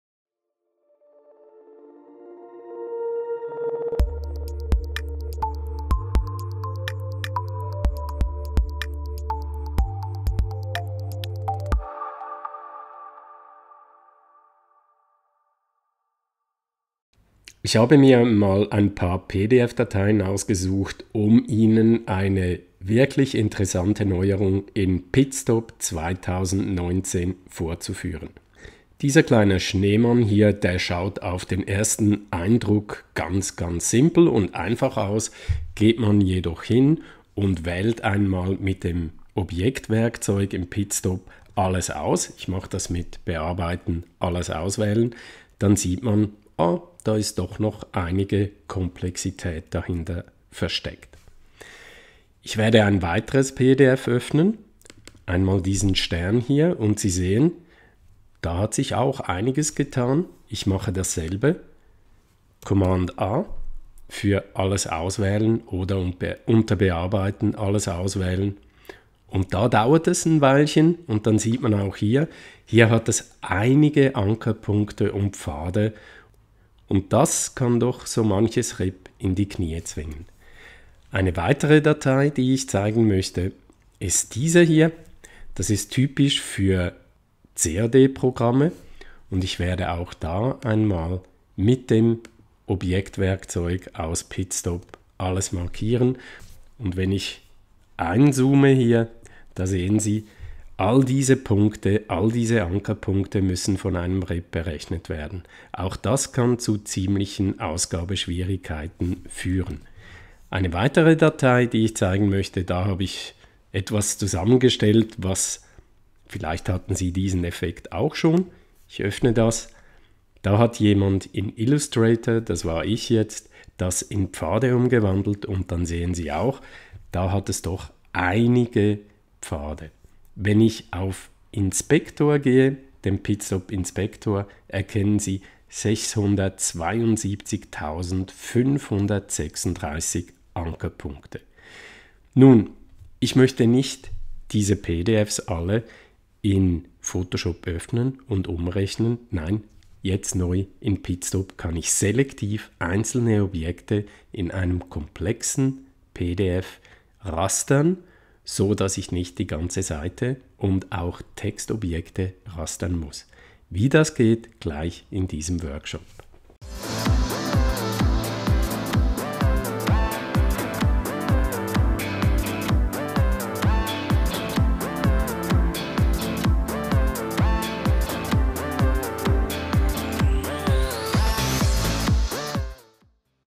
PitStop 2019: Objekte rastern Einleitung
Mit Enfocus PitStop 2019 ist es möglich mittels dem Objektbrowser einzelne Elemente auf einer PDF Seite zu rastern. Man muss nicht mehr die ganze Seite in Photoshop öffnen, sondern kann die Objekte mit dem Objektbroser auswählen um sie gezielt und mit gewünschten Einstellungen zu rastern.